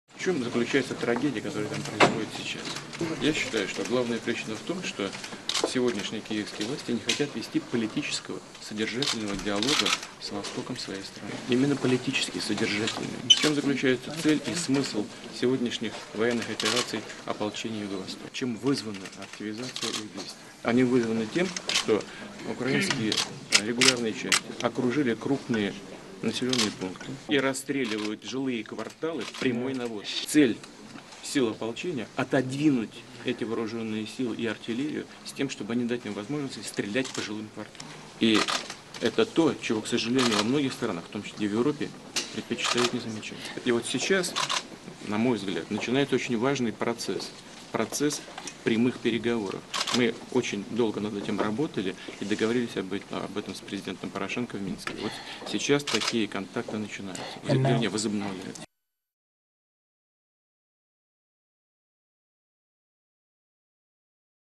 Путин рассказал журналисту Би-би-си об Украине - BBC Russian
В ходе визита президента России Владимира Путина в Якутск к нему подошел журналист BBC Джон Суини и неожиданно спросил его о ситуации на Украине. Высказав свою точку зрения, Владимир Путин пожелал журналисту всего хорошего и, не выслушав следующий вопрос, удалился.